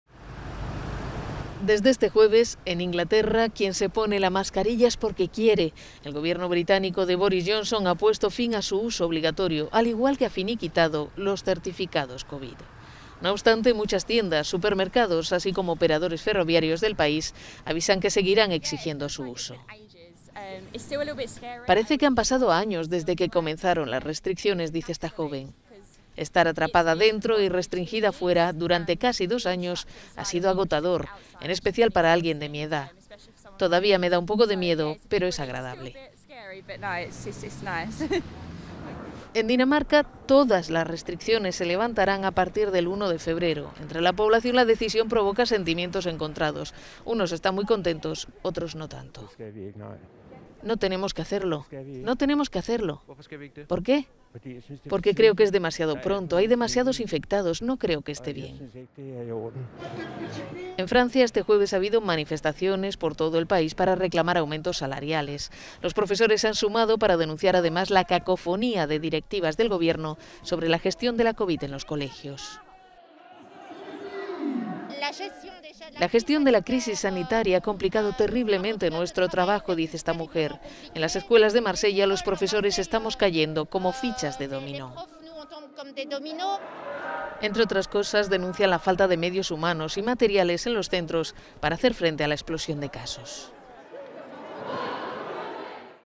COVID-19 | ¿Hacia el fin de las restricciones en Europa? Reino Unido y Dinamarca abren el camino
COVID-19 | ¿Hacia el fin de las restricciones en Europa? Reino Unido y Dinamarca abren el camino para volver a una "completa normalidad"